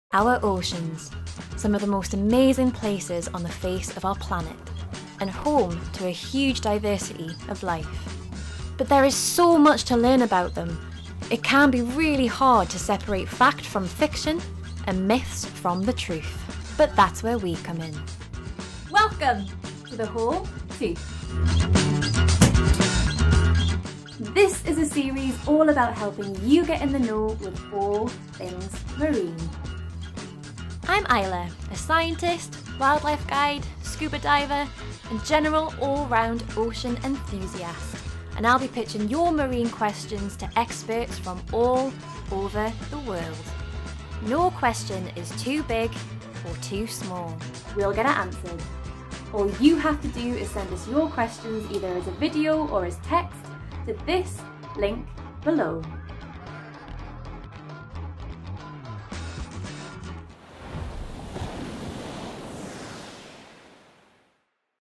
The Whole Tooth Trailer
The Whole Tooth - new vlog COMING SOON!

A fun new series where we get your marine questions answered by ocean experts!

No question is too big or too small - our host Dr Isla Hodgson has you covered.

If you want to know something about sharks, rays, turtles, corals, or anything else in our oceans, be sure to submit your question at the link below (where we accept video submissions) or in the comments.